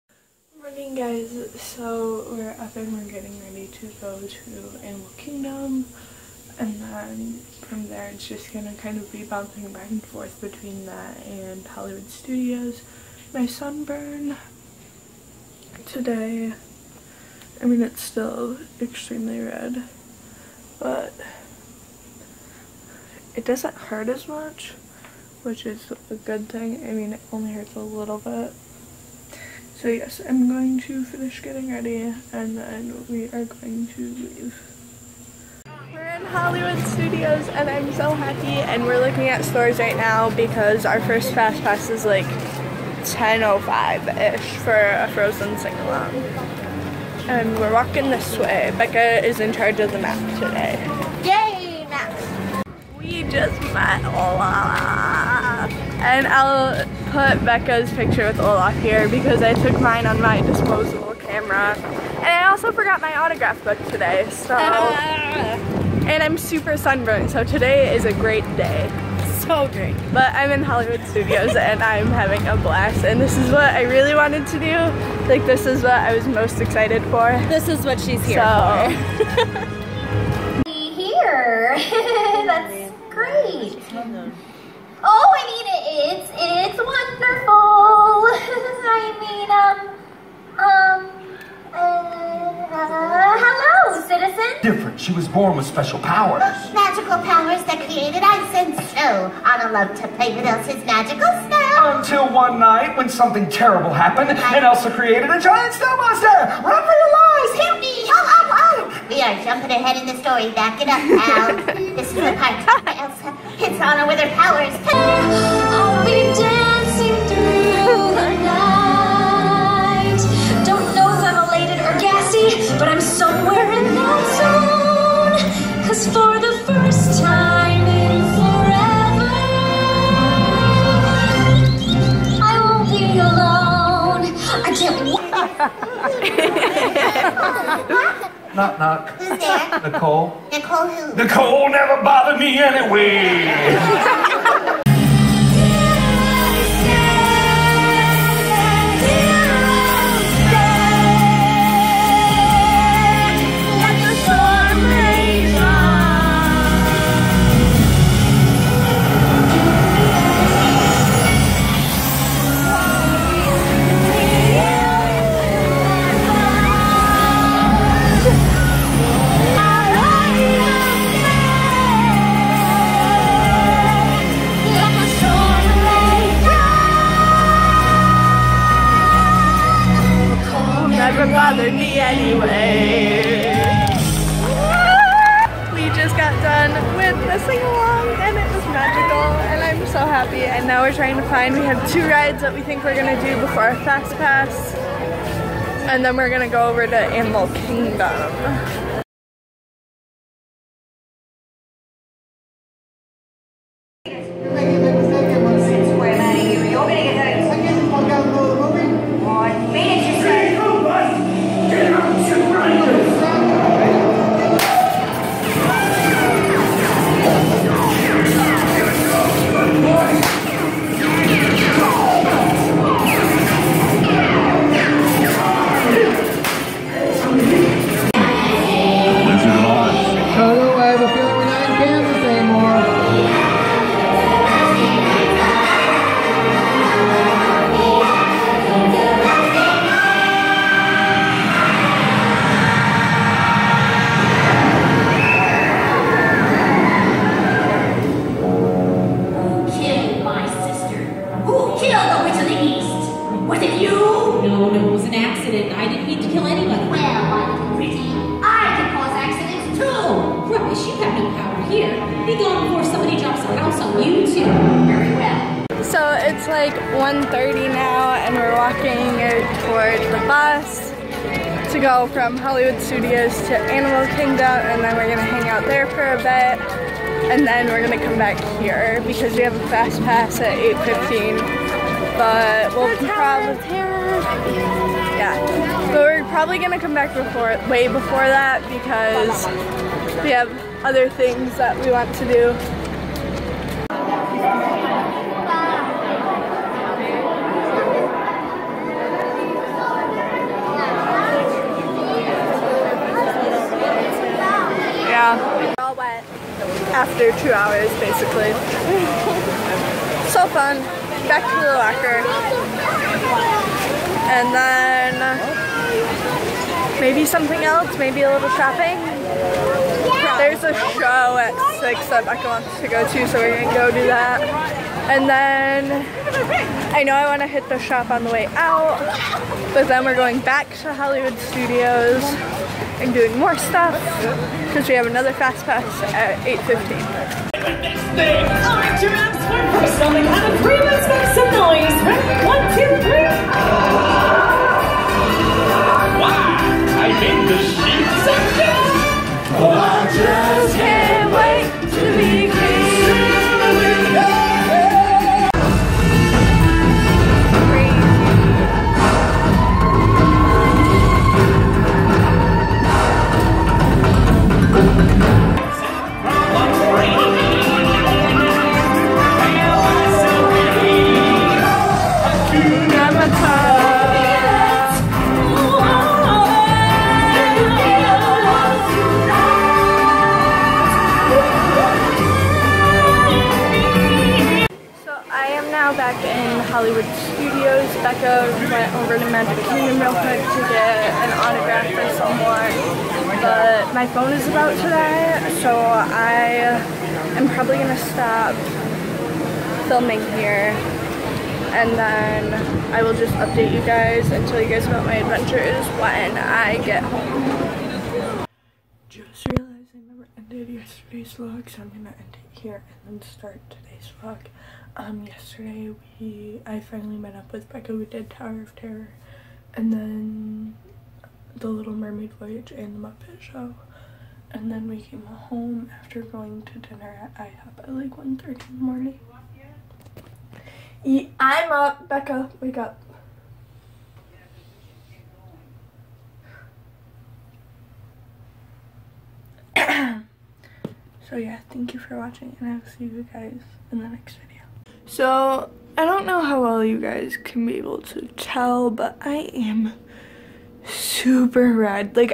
MY FAVORITE DAY (Disney Day 3)
Spent the day between Hollywood Studios and Animal Kingdom. This was my favorite disney day because my mom loves Hollywood Studios so I felt closer to her, and I quickly fell in love with it which made me get more excited and involved in whatever we did.